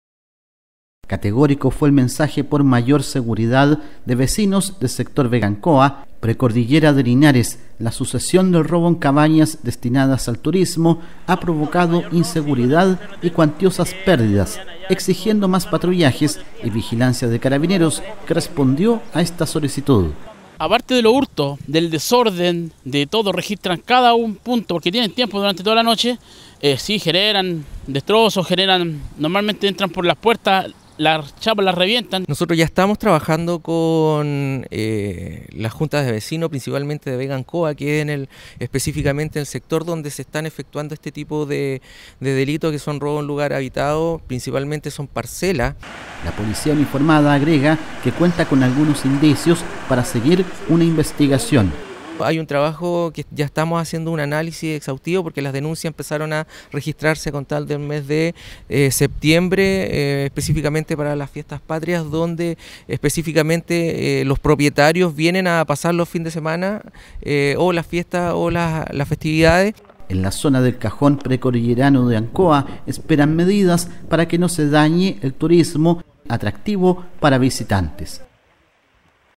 Carabineros por robos en Embalse Ancoa
Categórico fue el mensaje por mayor seguridad, de vecinos del sector Vega Ancoa, precordillera de Linares. La sucesión del robo en cabañas dedicadas al turismo, ha provocado inseguridad y cuantiosas pérdidas.
Exigiendo más patrullajes y vigilancia de Carabineros. Que respondió a esta solicitud.
La policía uniformada, agrega que cuenta con algunos indicios para seguir la investigación.
En la zona del cajón precordillerano de Ancoa, esperan medidas, para que no se dañe el turismo en una zona de atractivo para visitantes.